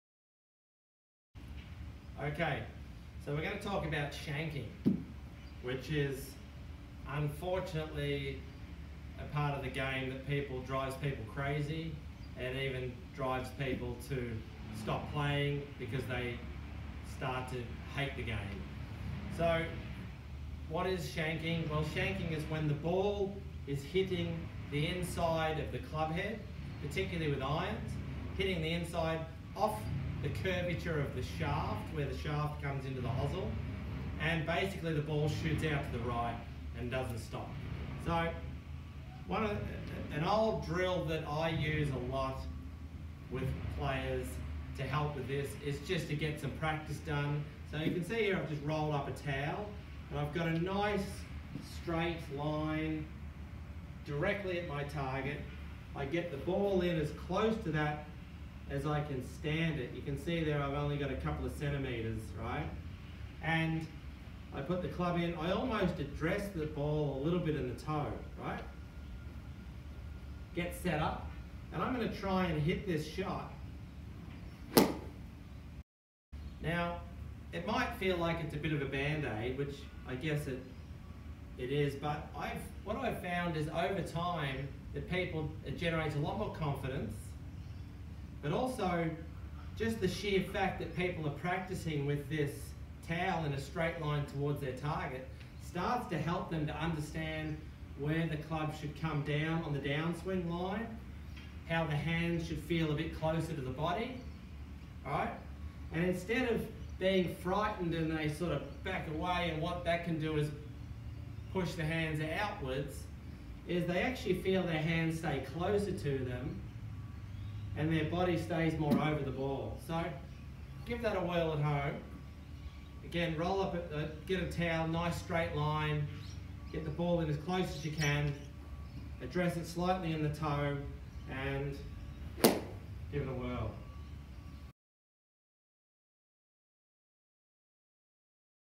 Golf Skills At Home With Craig Spence - Shanking
A lot of people struggle with shanking, in this video I talk about the best drill you can do if your struggling with it. Trust me, when you need to just feel the club face on the ball this is a great drill. And can actually help the path of your club head also. Good luck.